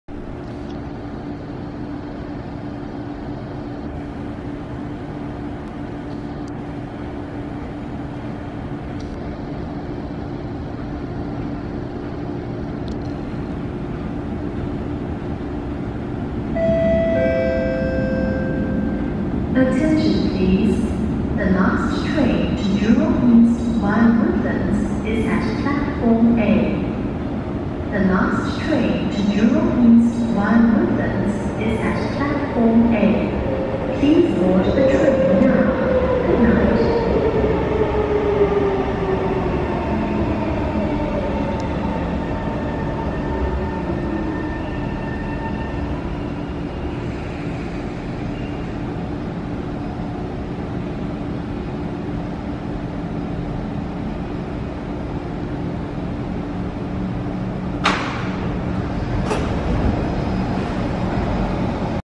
[Last Train] C151 033/034 Arriving Marina South Pier (Northbound)
C151 033/034 arriving Marina South Pier. Last train bound for Jurong East. 18 April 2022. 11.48 pm.